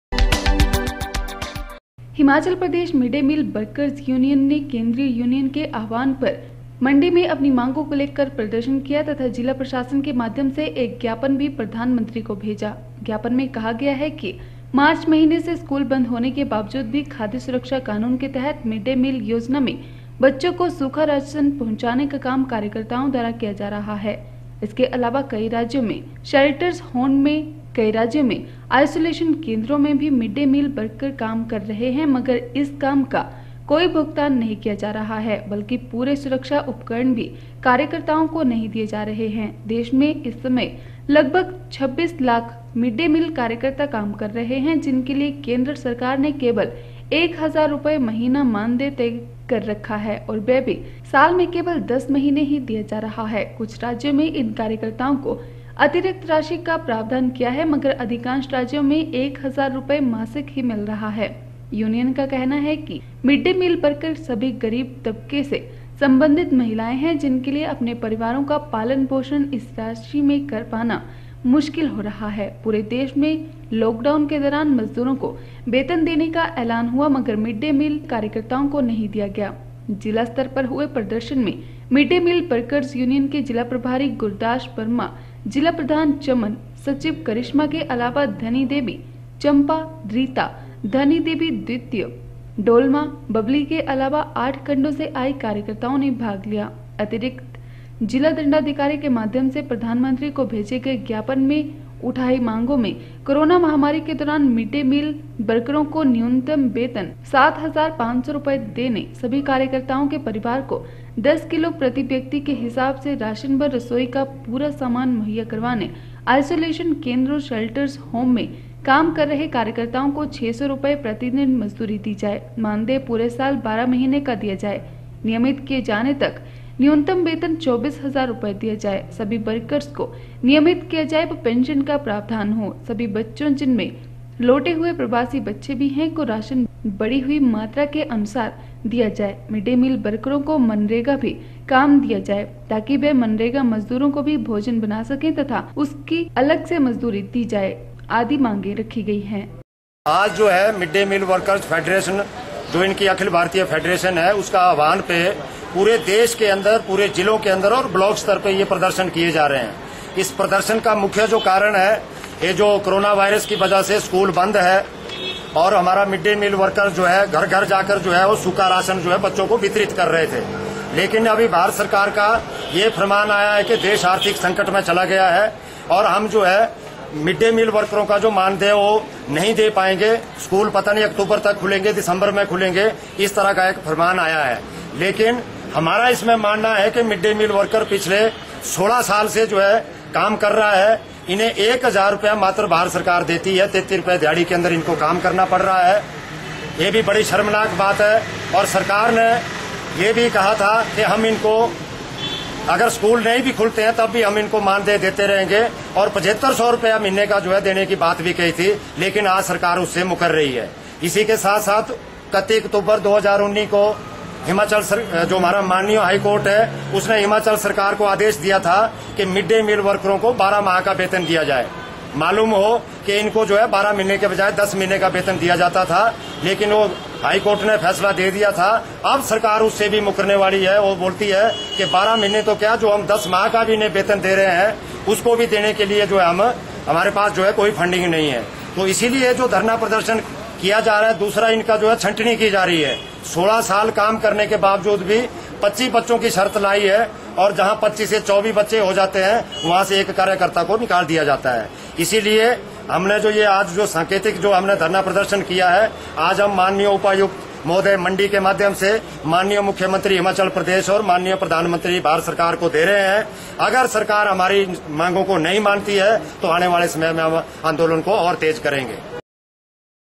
27 june 10 मिड डे मील कार्यकर्ताओं ने किया प्रदर्शन, प्रधानमंत्री को भेजा ज्ञापन
मंडी, 26 जून। हिमाचल प्रदेश मिड डे मील वर्करज यूनियन (संबंधित सीटू) ने केंद्रीय यूनियन के आह्वान पर शुक्रवार को मंडी में अपनी मांगों को लेकर प्रदर्शन किया तथा जिला प्रशासन के माध्यम से एक ज्ञापन भी प्रधानमंत्री को भेजा। ज्ञापन में कहा गया कि मार्च महीने से स्कूल बंद होने के बावजूद भी खाद्य सुरक्षा कानून के तहत मिड डे मील योजना में बच्चों को सूखा राशन पहुंचाने का काम कार्यकर्ताओं द्वारा किया जा रहा है, इसके अलावा कई राज्यों में शैल्टर्स होम में, कई राज्यों में आईसोलेशन केंद्रों में भी मिड डे मील वर्कर काम कर रहे हैं मगर इस काम का  कोई भुगतान नहीं किया जा रहा है बल्कि पूरे सुरक्षा उपकरण भी कार्यकर्ताओं को नहीं दिए जा रहे हैं।